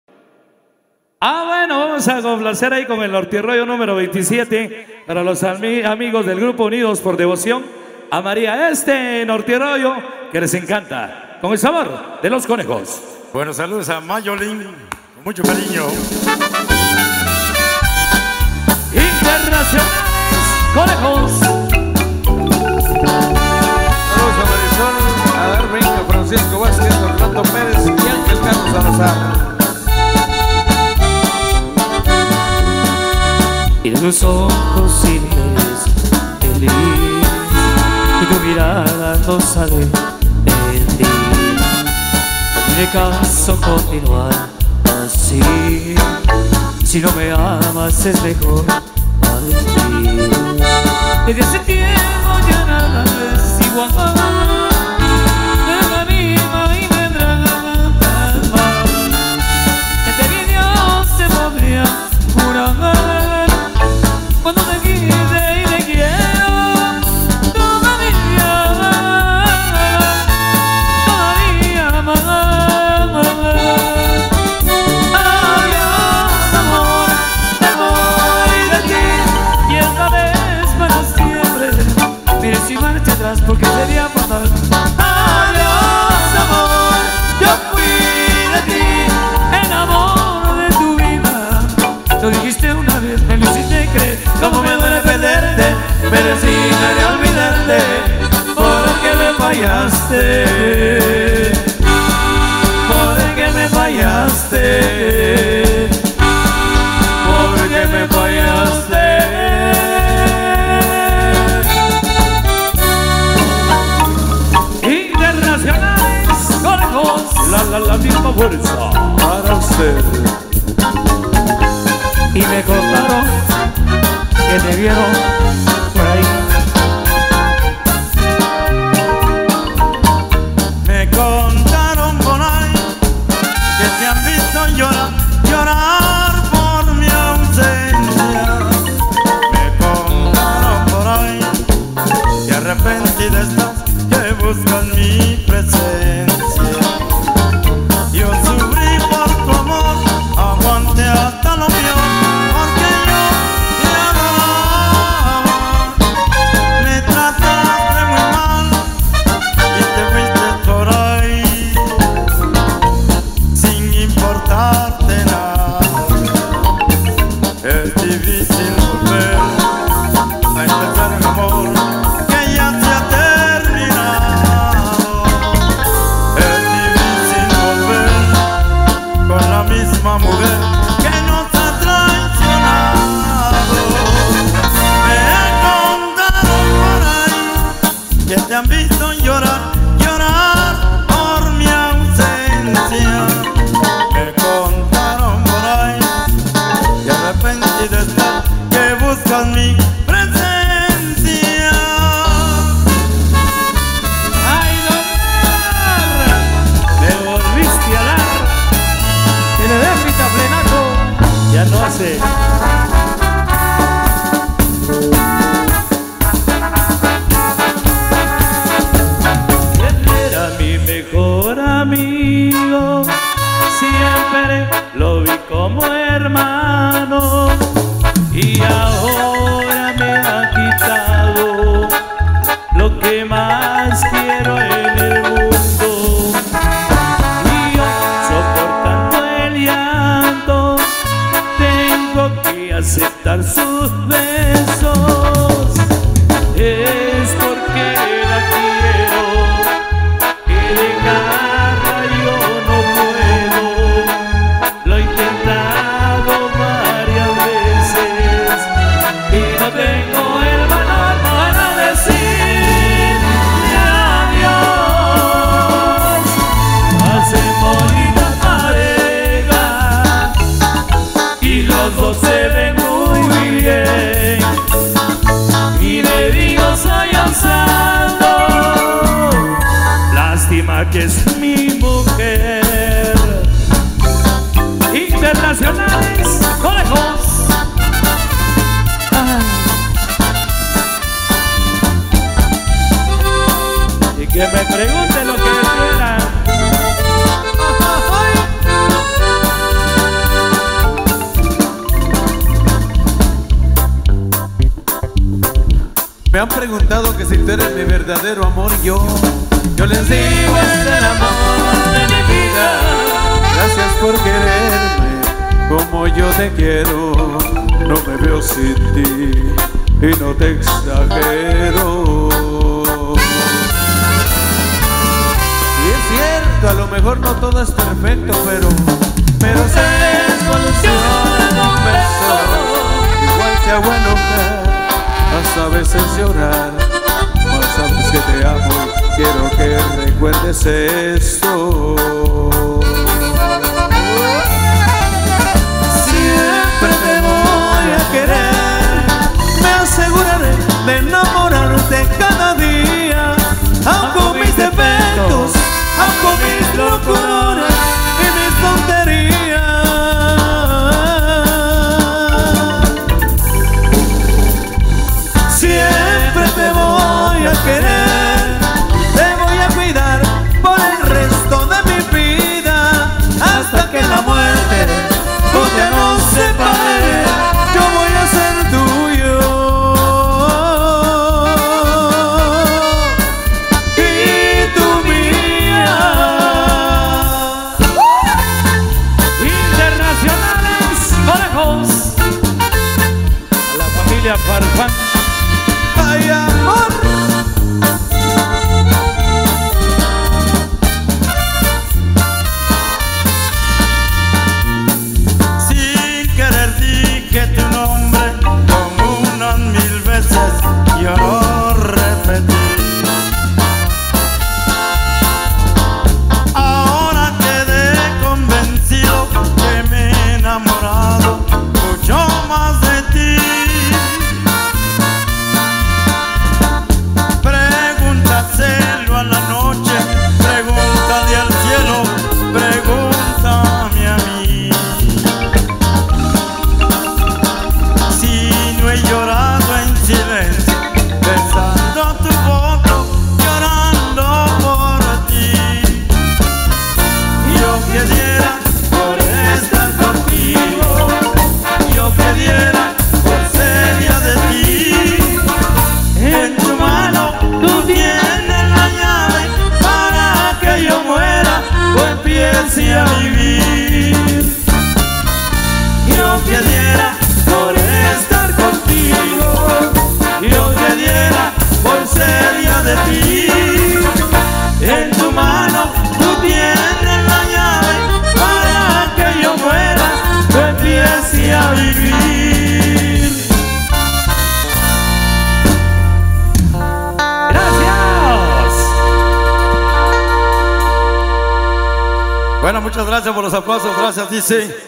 GRUPO UNIDOS POR DEVOCION A MARIA 2019 - 19
CONCIERTO SEPTIMA EDICION, GRUPO UNIDOS POR DEVOCION A MARIA 2019, CIUDAD VIEJA, SACATEPEQUEZ, GUATEMALA, C.A., AMENIZADO POR INTERNACIONALES CONEJOS, CHARLY PRODUCTIONS INTERNATIONAL